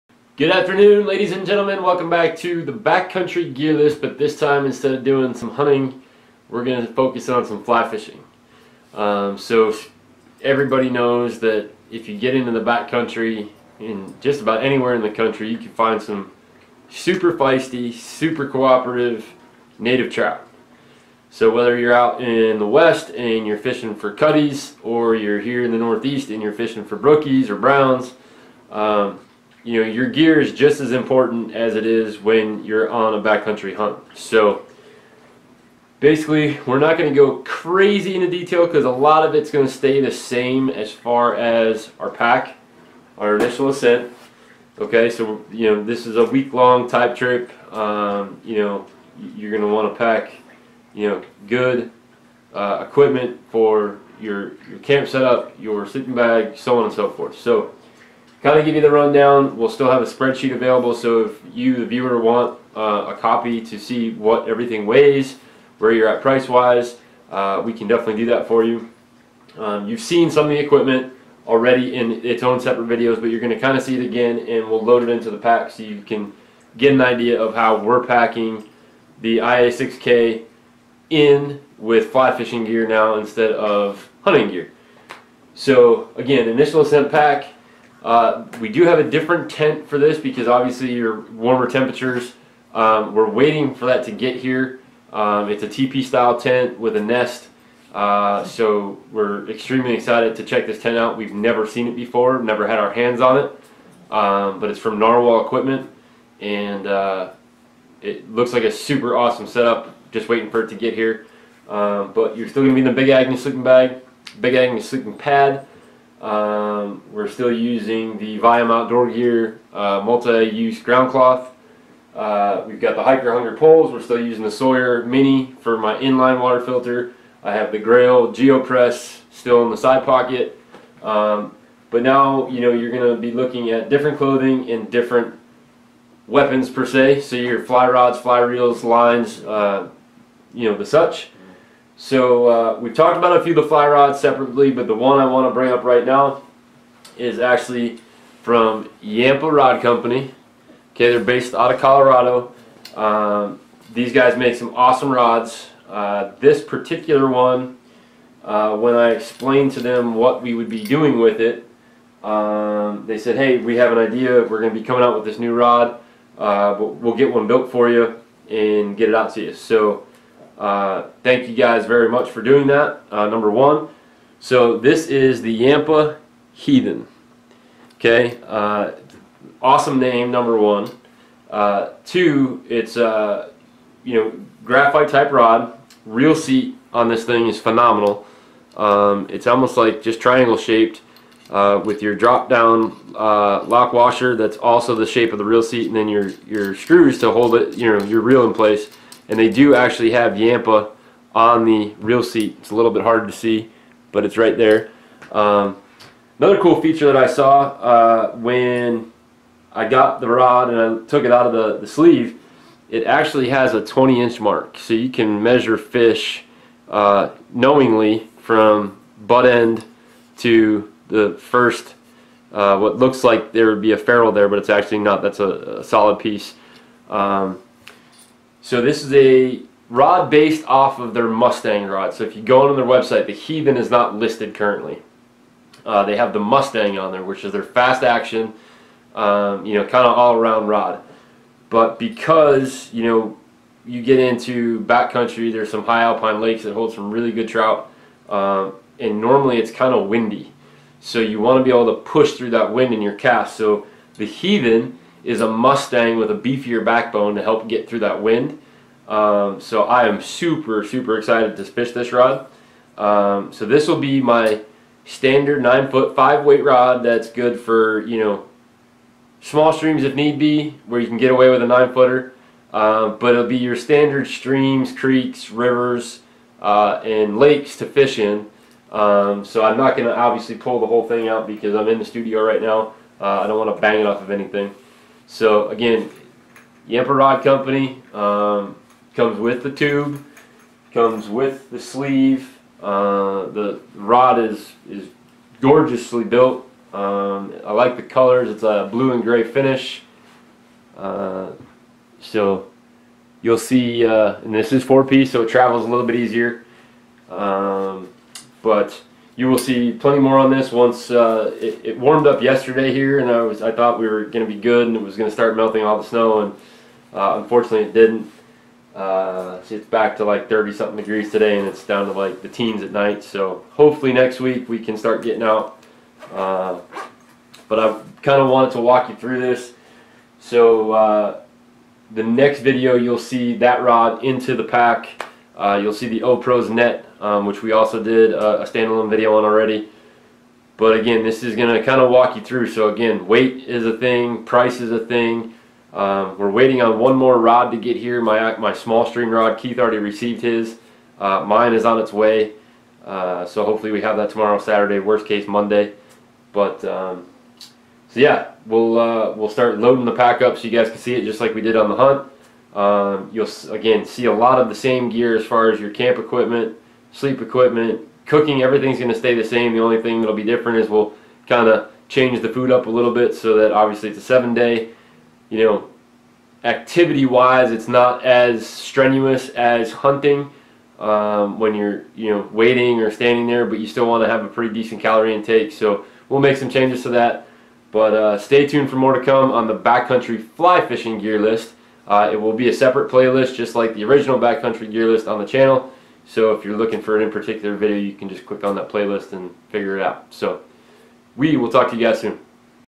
Rack and Rod Chronicles BC Gear List for Fly Fishing Yampa Fly Rods Heathen 9' 5 Weight
You can't just find this rod, at least not yet!! But if you call out to the awesome folks and Yampa and ask for one, well I am sure they would be happy to help. However the Heathen was born from their Mustang Rod, almost Identical, except the Heathen was a bit more of gym rat.